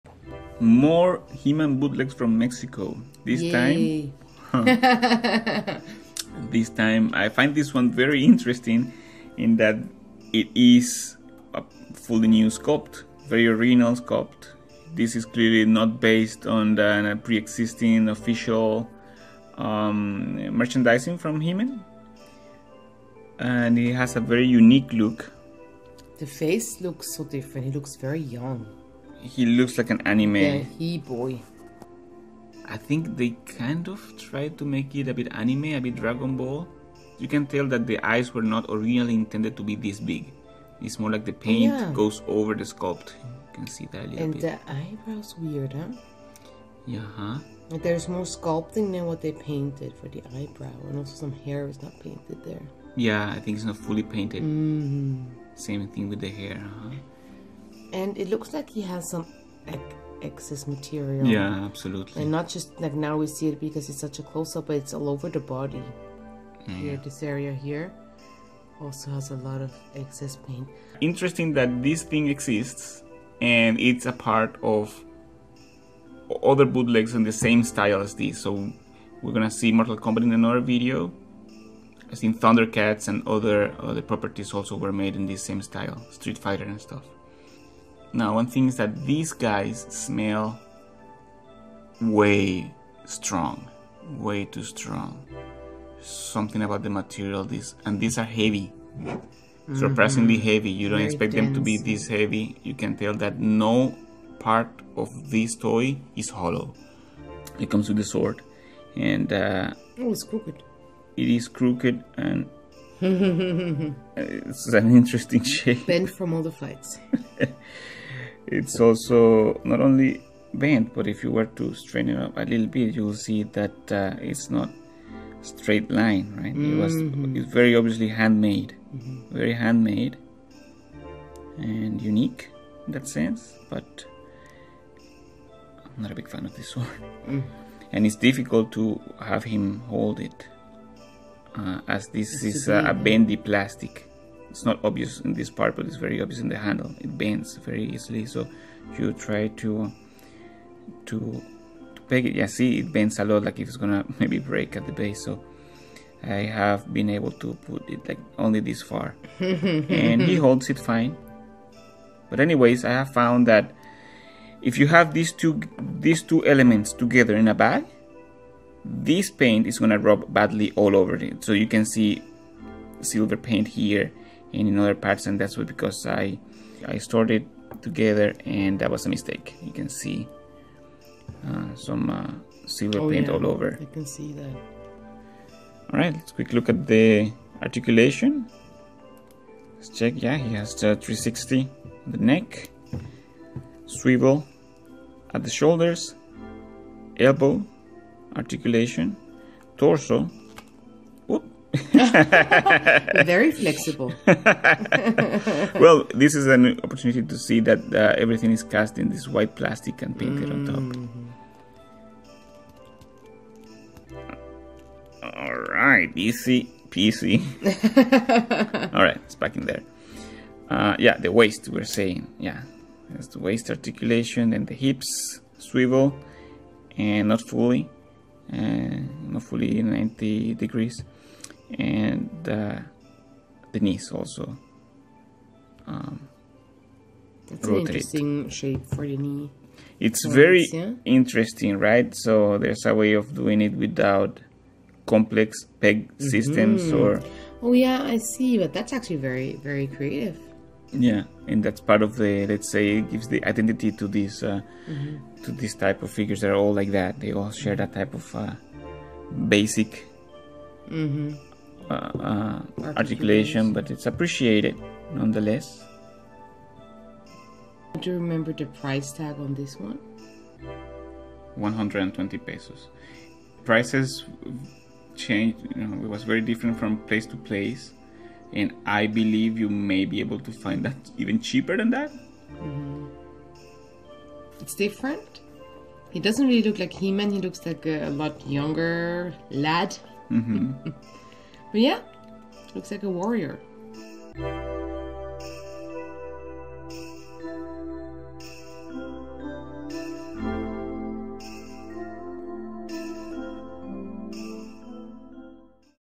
He-Man Masters of the Universe bootleg action figure from Mexico
"He-Boy" figure review.